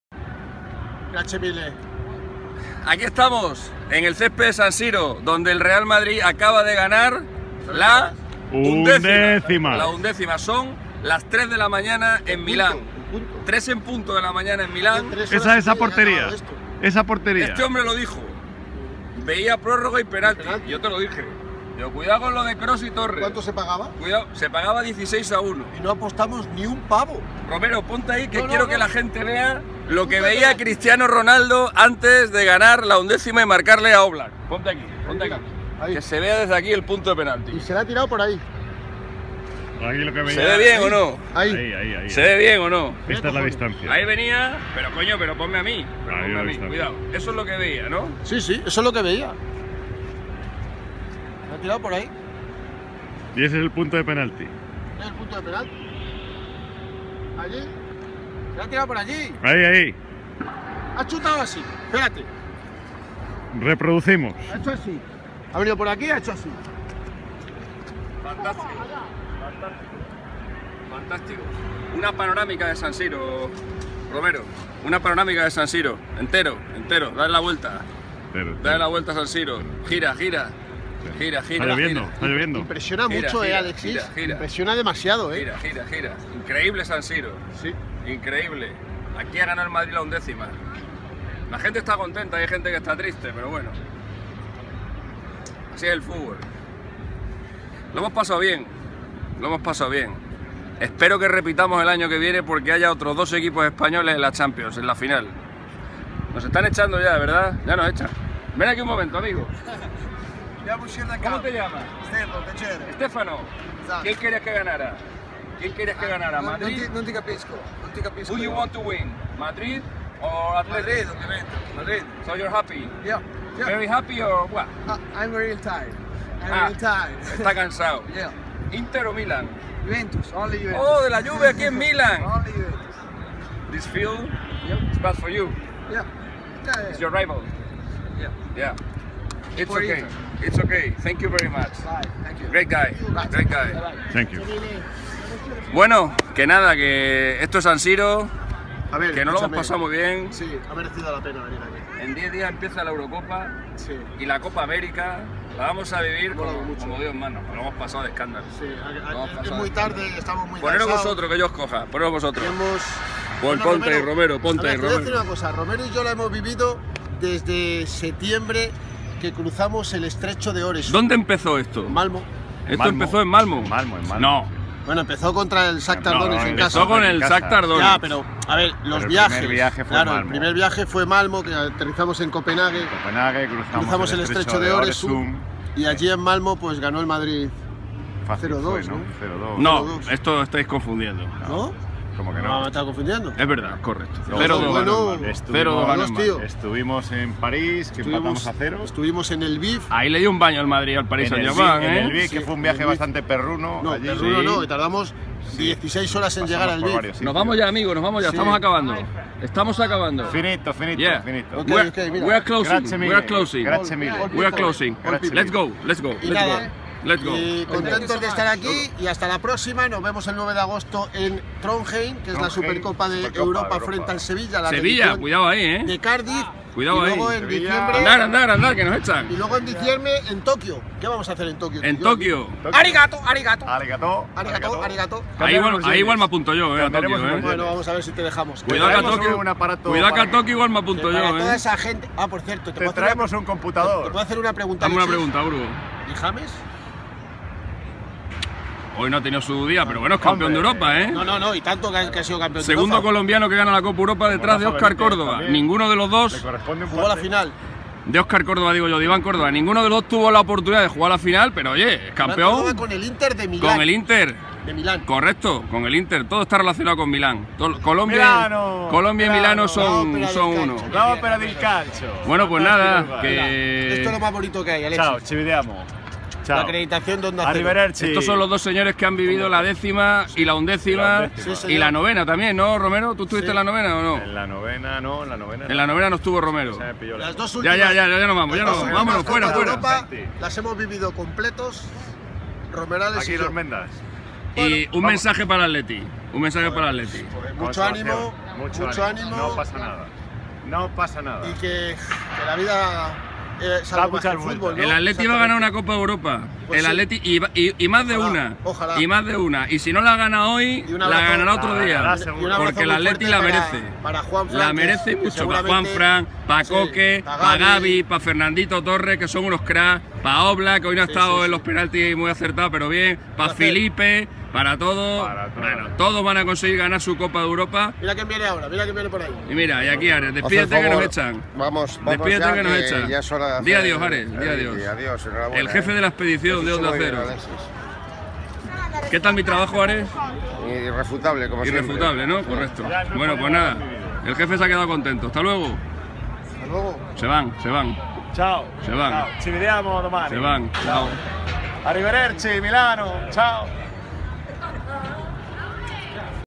MisterChip & friends en San Siro (parte 2)
Desde el césped nada más concluir la final de la Champions League 2015-16